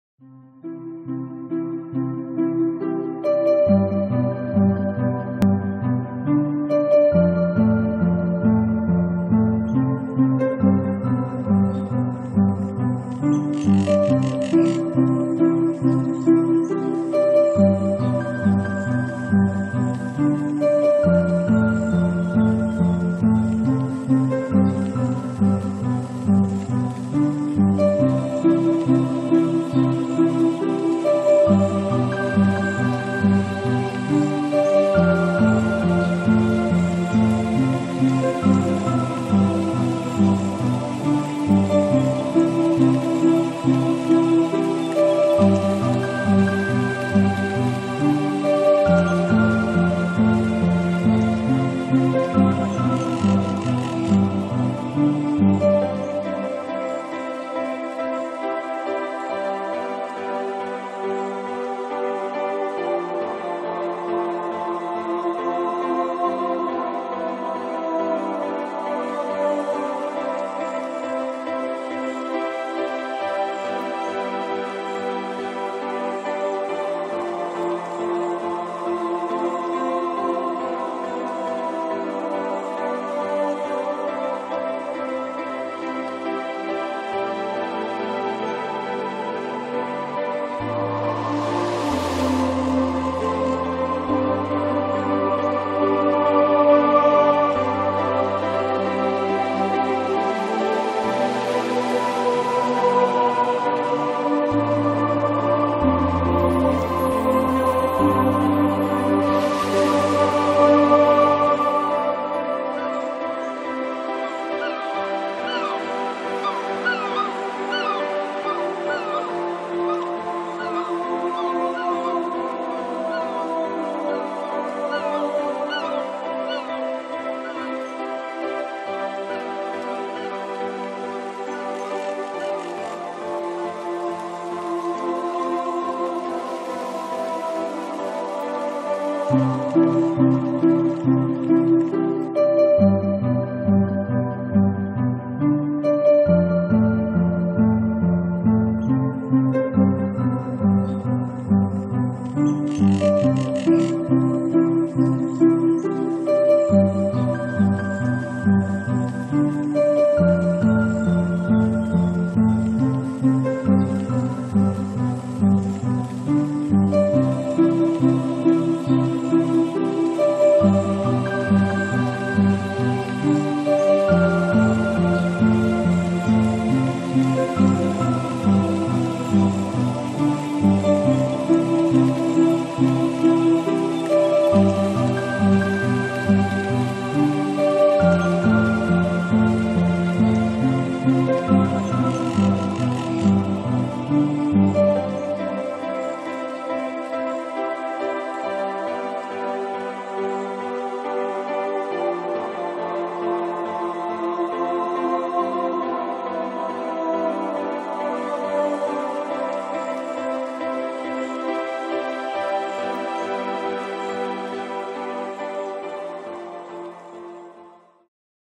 Timelapse Cam 1 - 29. august 2022
Været i Moss mandag 29. august 2022.